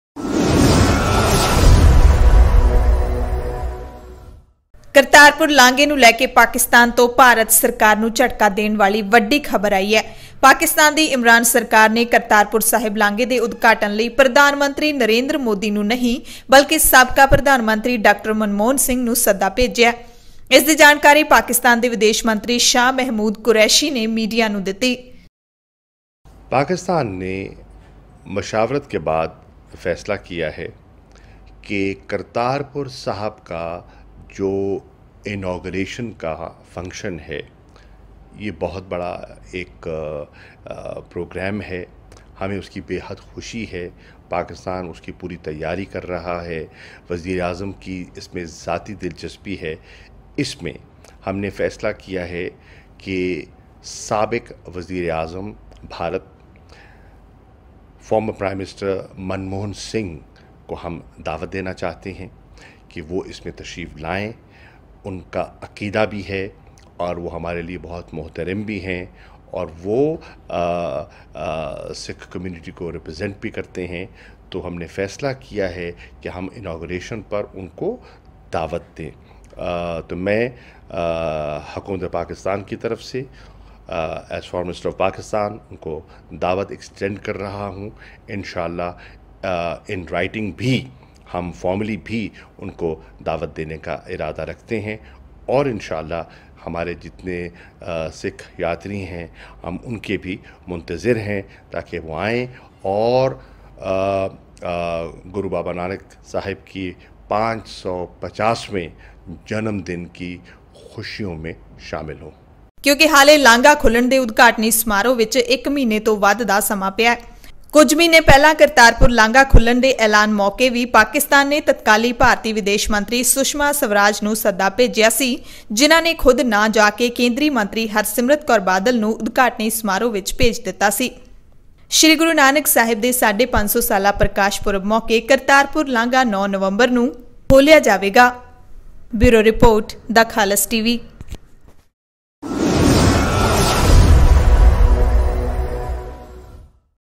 Pakistan Govt invites former PM Dr. Manmohan Singh for Kartarpur corridor inauguration ll KHALAS TV
We are your voice. We are for Human rights.